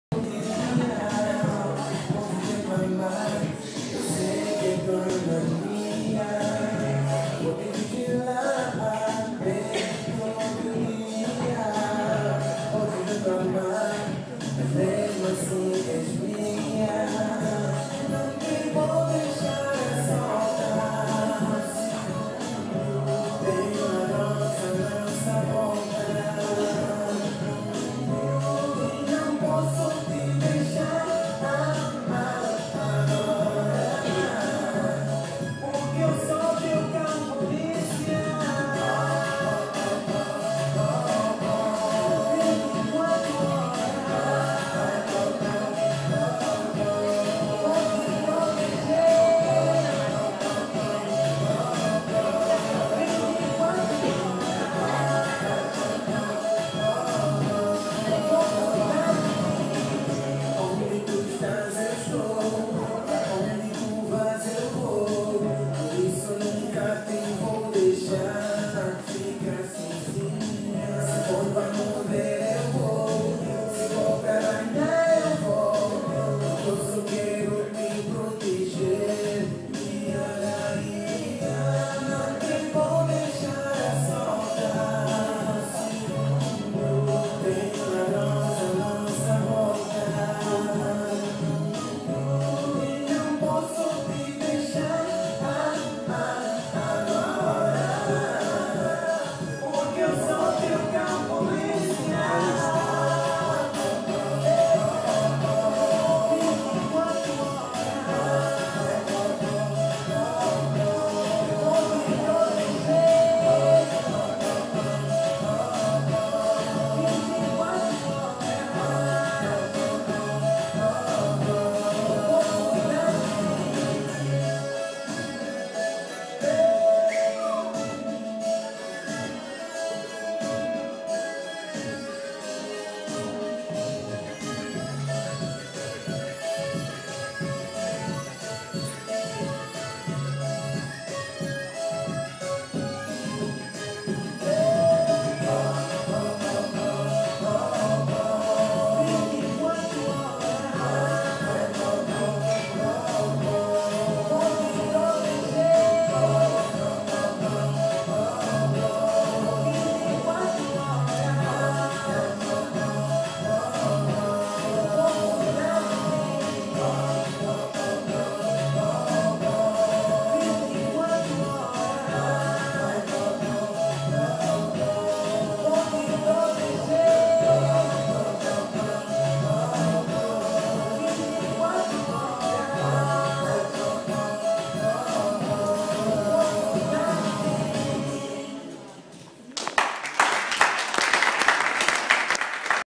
3° Stage Kizomba con Fernando Bum Bum e Michela Vernati.MOV
livello:  intermedio
luogo: Zero Club Cagliari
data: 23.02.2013

Figure presentate:
1. base 1
2. base 2
3. base 3
4. base 4
5. pasada da 3
6. pasada da 5
7. uscita uomo
8. uscita donna
9. pasada laterale